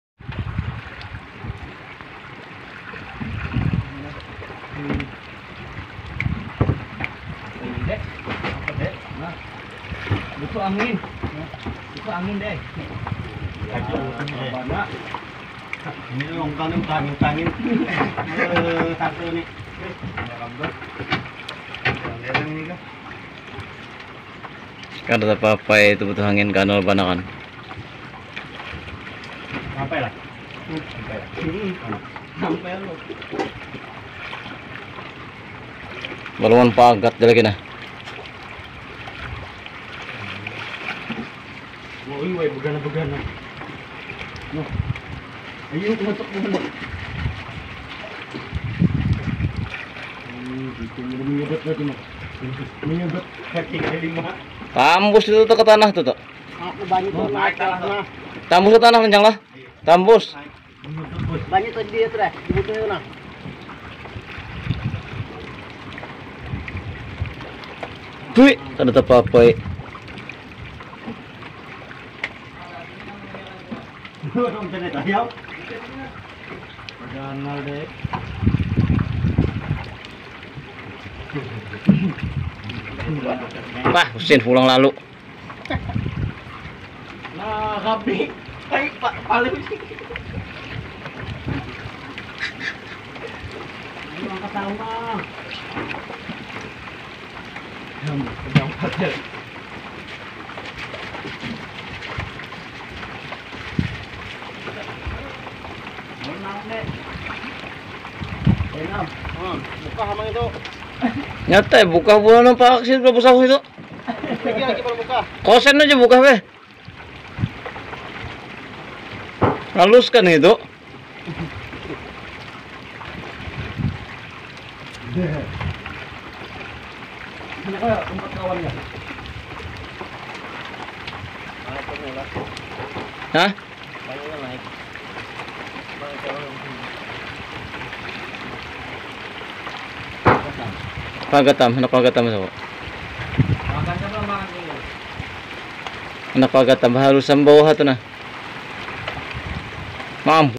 DETIK-DETIK ANGIN TOPAN MUNCUL DILAUTAN SAAT NELAYAN MENARIK JARING
untuk video kali ini saya mau sharing video momen paling langka yang terjadi di lautan, detik" muncul nya angin topan di lautan dekat dengan perahu om saya